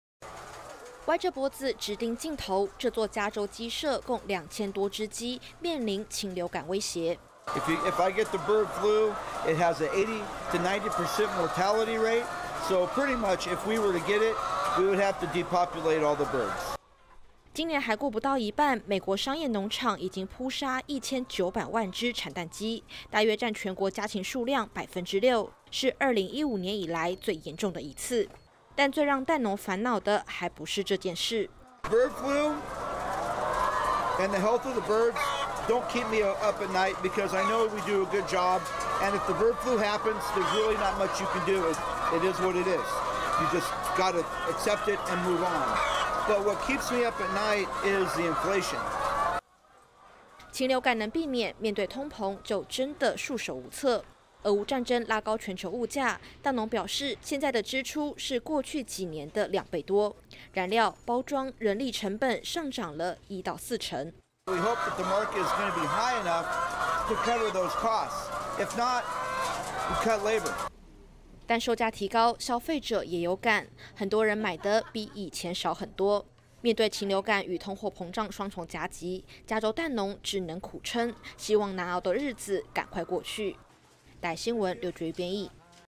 美國爆發禽流感 已撲殺1900萬隻蛋雞
美國加州蛋農，現在面臨禽流感與通貨膨脹的雙重威脅。禽流感致死率高，美國商業農場今年撲殺產蛋雞的數量，是7年以來最高的一次。另外，飛漲的物價，更壓得蛋農快喘不過氣，他們表示，現在的支出是過去幾年的兩倍多，只能靠提高售價，努力打平成本。

歪著脖子、直盯鏡頭，這座加州雞舍，共兩千多隻雞，面臨禽流感威脅。

加州蛋農 希爾克：「如果禽流感爆發，死亡率大概是80%到90%，所以基本上 只要我們中了，就不得不減少所有家禽的數量。」

今年還過不到一半，美國已經撲殺1900萬隻產蛋雞，大約占全國家禽數量6%，是2015年以來最嚴重的一次。但最讓蛋農煩惱的，還不是這件事。

加州蛋農 希爾克：「禽流感或雞的健康狀況，不會讓我睡不著覺，因為我知道我(預防工作)做得很好，如果禽流感爆發了，你也做不了什麼 事情就是這樣，你只能接受 然後繼續前進，真正讓我輾轉難眠的是通貨膨脹。」

禽流感能避免，面對通膨，就真的束手無策。俄烏戰爭拉高全球物價，蛋農表示，現在的支出，是過去幾年的兩倍多，燃料、包裝、人力成本上漲了一到四成。

加州蛋農 希爾克：「我們希望市場價格，能高到打平這些開銷，再不行的話 就得裁員。」

但售價提高，消費者也有感，很多人買得比以前少很多。面對禽流感與通貨膨脹雙重夾擊，加州蛋農只能苦撐，希望難熬的日子，趕快過去。

RTV 劉玨妤 編譯 卓谷翰 剪輯